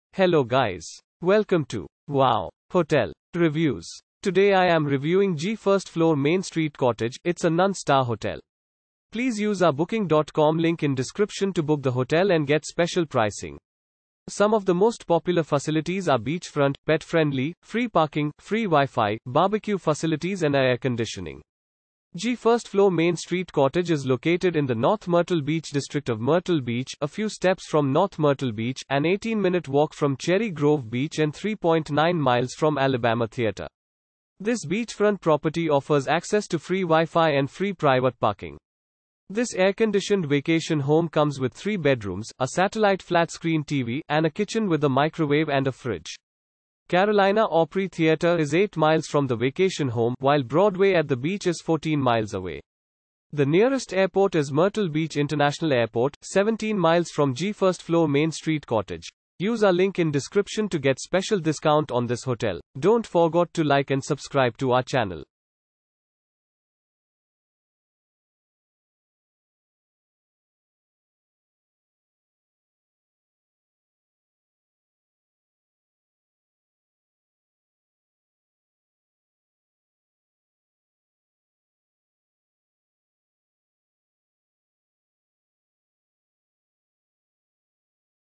G 1st Floor Main Street Cottage Review - Myrtle Beach , United States of America
G 1st Floor Main Street Cottage

G 1st Floor Main Street Cottage is located in the North Myrtle Beach district of Myrtle Beach, a few steps from North Myrtle Beach, an 18-minute walk from Cherry Grove Beach and 3.9 miles from Alabama Theater. This beachfront property offers access to free WiFi and free private parking.
This air-conditioned vacation home comes with 3 bedrooms, a satellite flat-screen TV, and a kitchen with a microwave and a fridge.
Carolina Opry Theater is 8 miles from the vacation home, while Broadway at the Beach is 14 miles away. The nearest airport is Myrtle Beach International Airport, 17 miles from G 1st Floor Main Street Cottage.